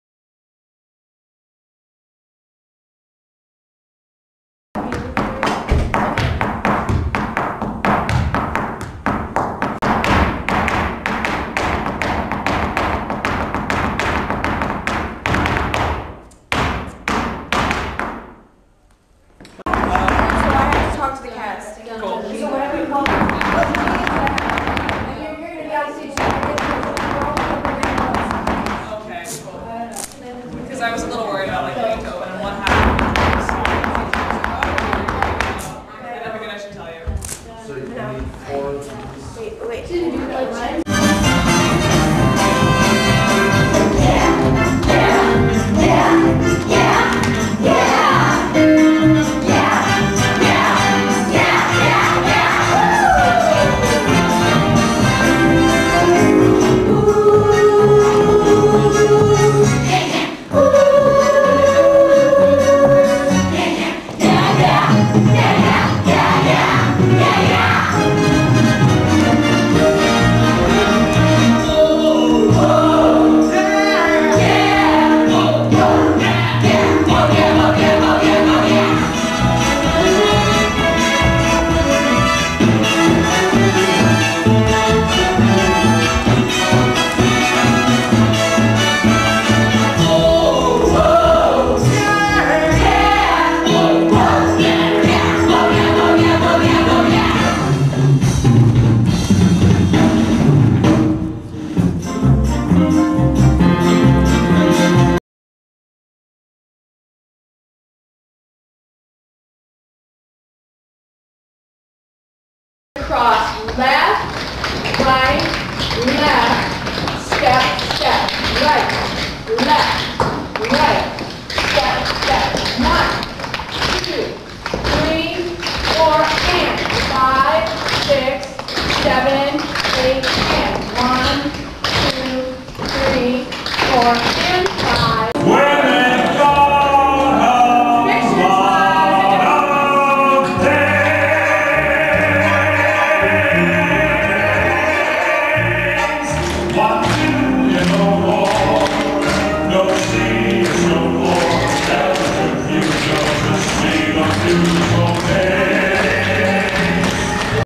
Horace Mann Dancing
Student-choreographed Dance Concert and 42nd Street audition.